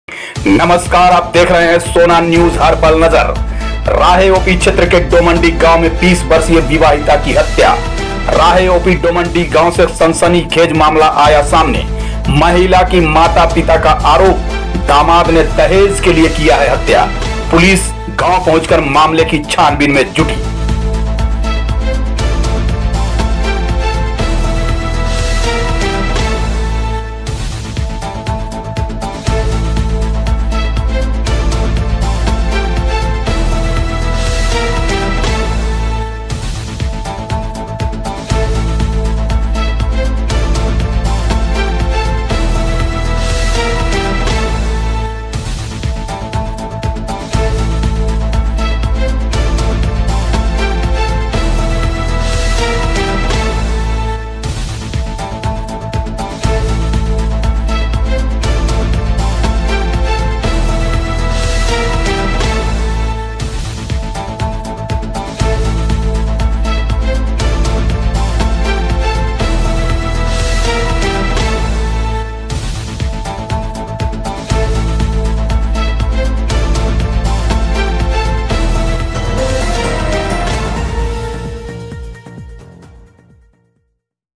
BREAKING राहे डोमनडीह गांव में 20 वर्षीय विवाहिता मृत पाई गई।।पुलिस जाँच में जुटी
BREAKING राहे ओपी क्षेत्र के डोमनडीह गांव में 20 वर्षीय विवाहिता की हत्या
राहे ओपी डोमन डीह गाँव से सनसनी खेज मामला आया सामने
महिला की माता -पिता का आरोप
दामाद ने दहेज के लिये किया है हत्या।
पुलिस गांव पहुँच कर मामले की छानबीन में जुटी